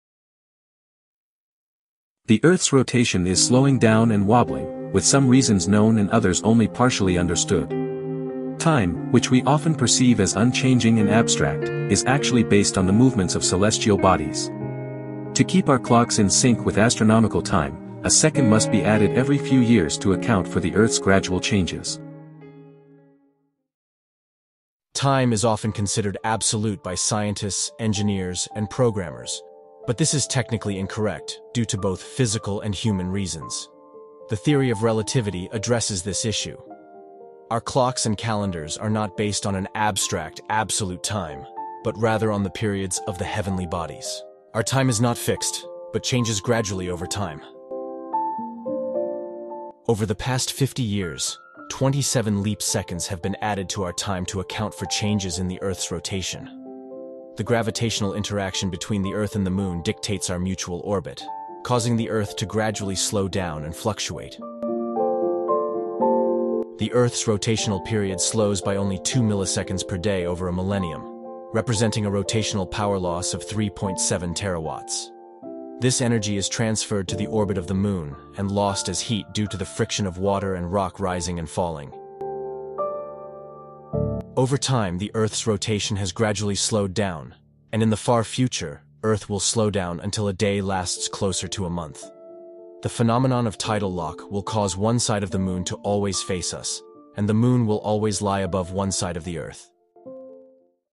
Is Earth's Rotation in Jeopardy?! Why We Need Leap Seconds NOW!
The Earth's rotation is slowing down and wobbling, with some reasons known and others only partially understood. Time, which we often perceive as unchanging and abstract, is actually based on the movements of celestial bodies. To keep our clocks in sync with astronomical time, a second must be added every few years to account for the Earth's gradual changes.